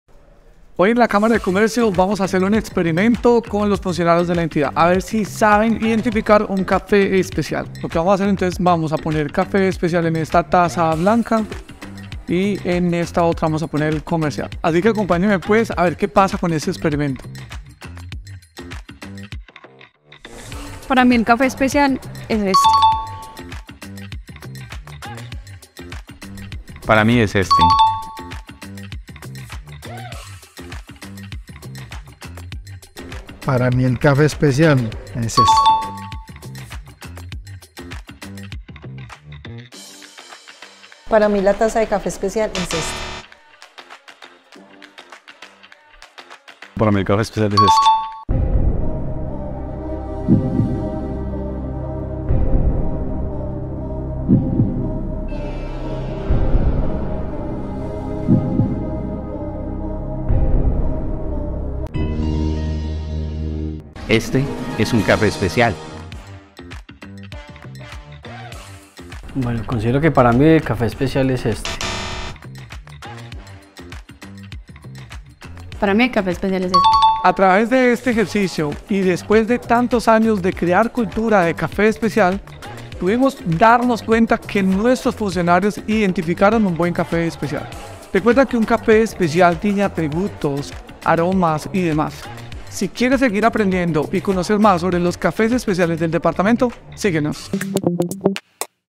¿Logras identificar el café especial?
En la Cámara de Comercio de Armenia y del Quindío pusimos a prueba la habilidad de nuestros funcionarios para identificar y diferenciar un café especial de uno comercial ☕🤔 ¿Cómo crees que les fue? ¡Dale play al video!